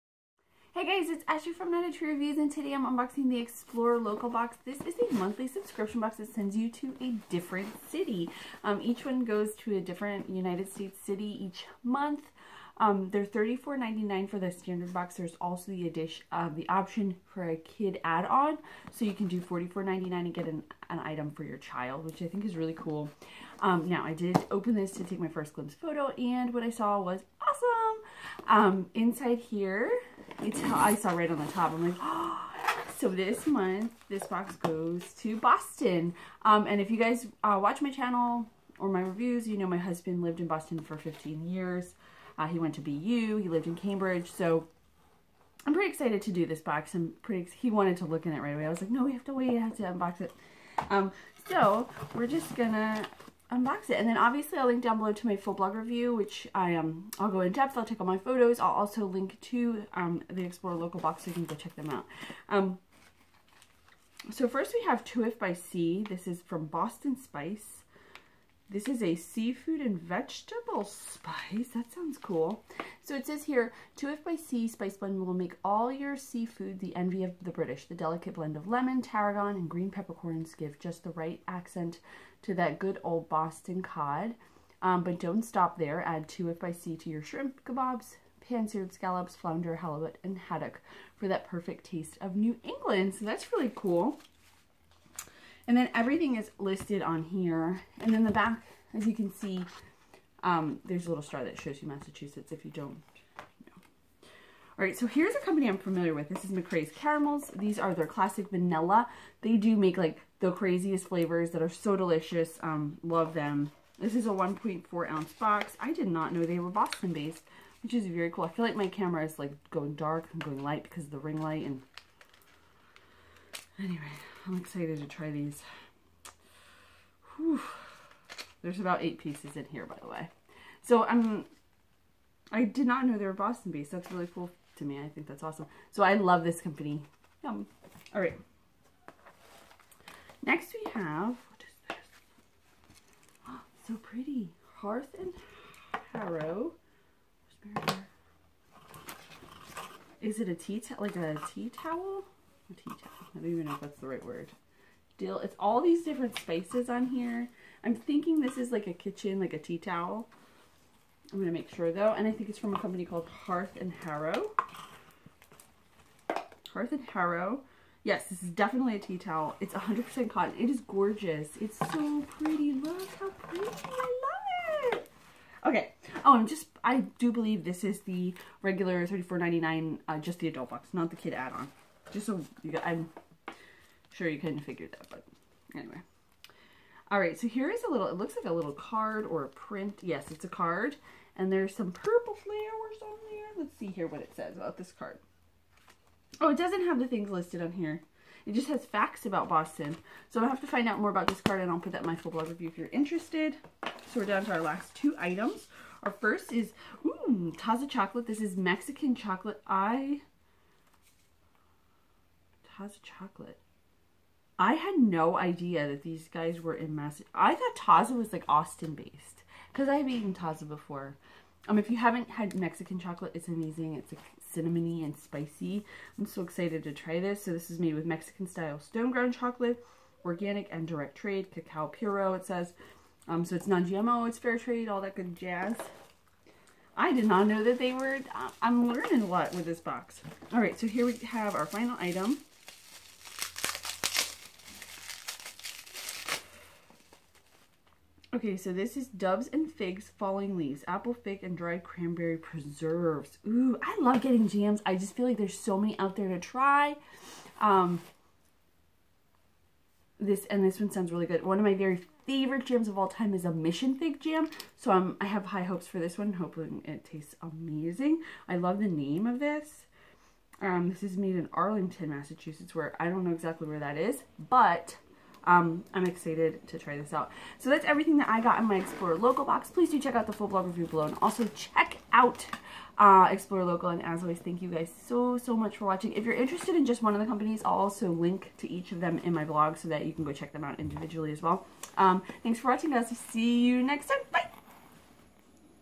Explore Local Box Unboxing | April 2017
Explore Local is a monthly box that takes you to a different US cit with their different foods and other artisanal items. Subscriptions are $34.99 and additionally there's a childs add on that sends on (or more) children's items for $44.99 monthly